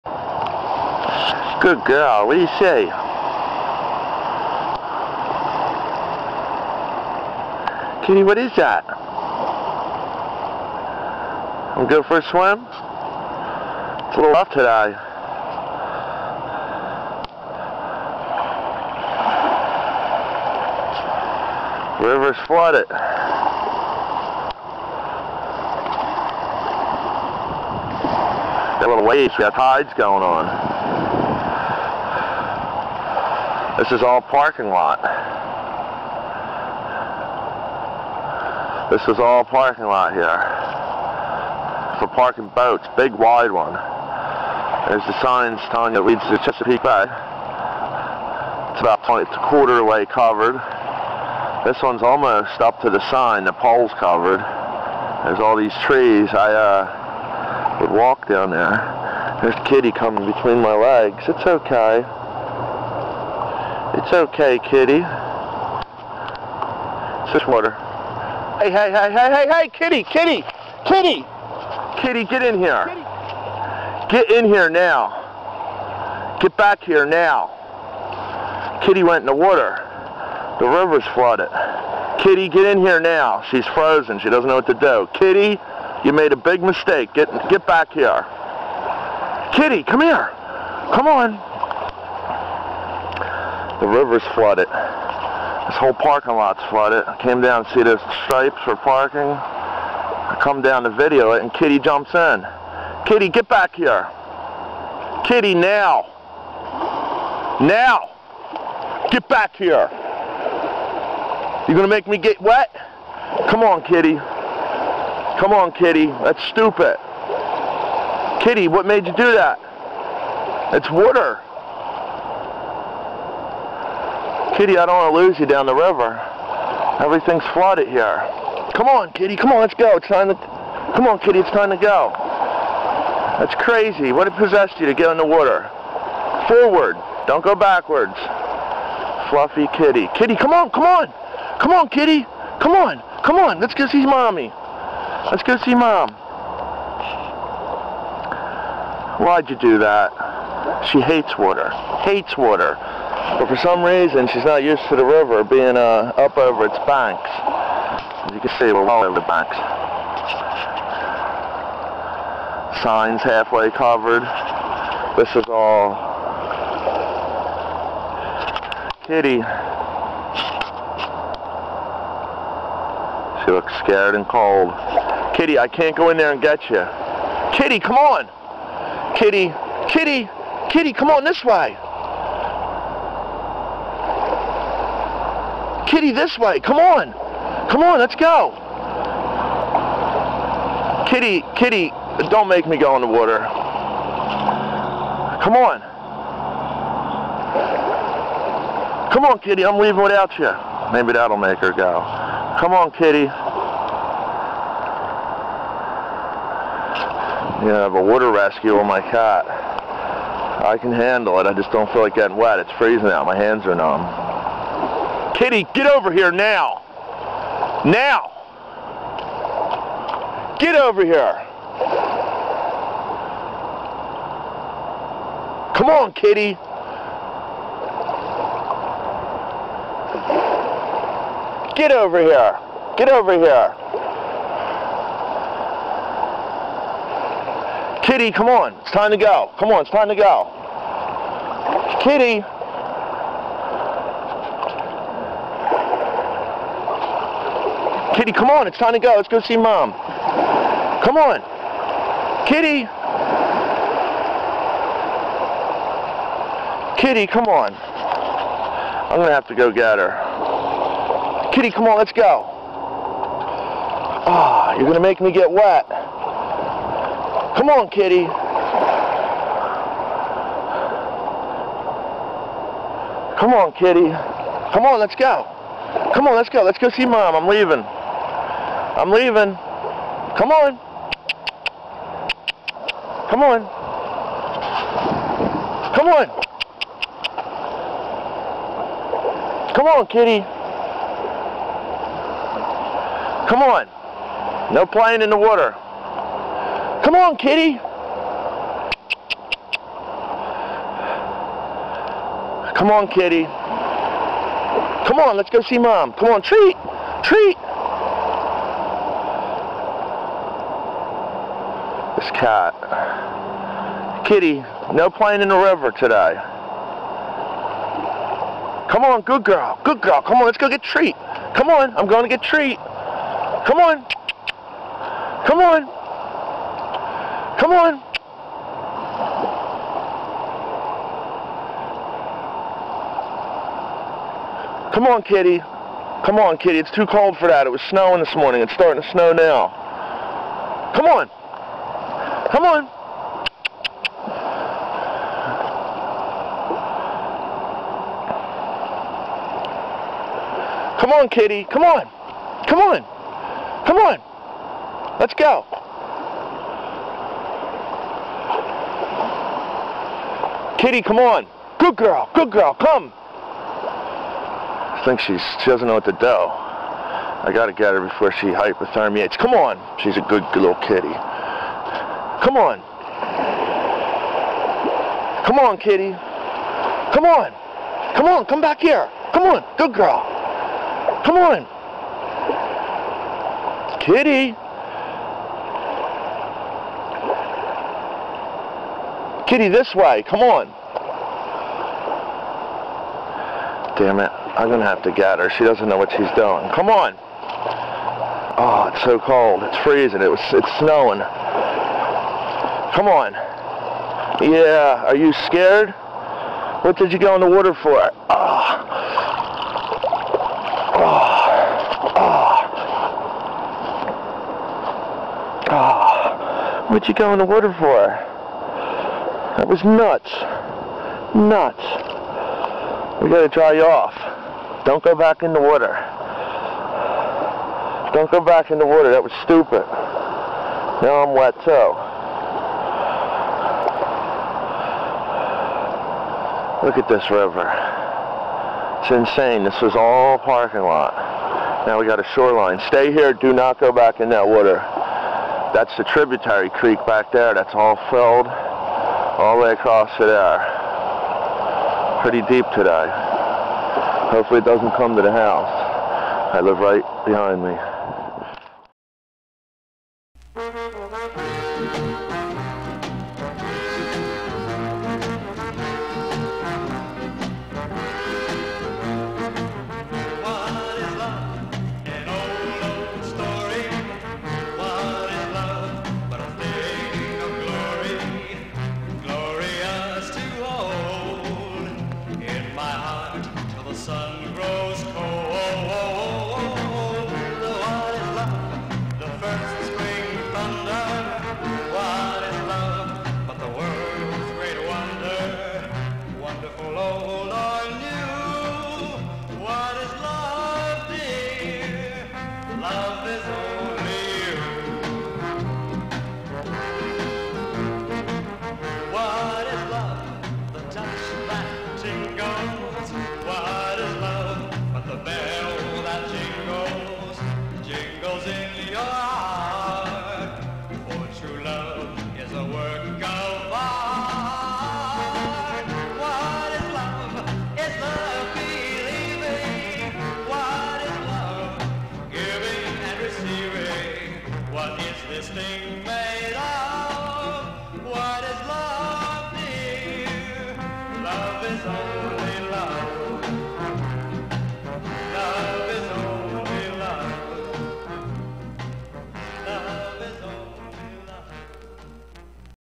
Cat in Flooded River
My cat decided to walk into the flooded banks of the Juniata River here in Central Pennsylvania today December 2 2010 with 21-feet water crest.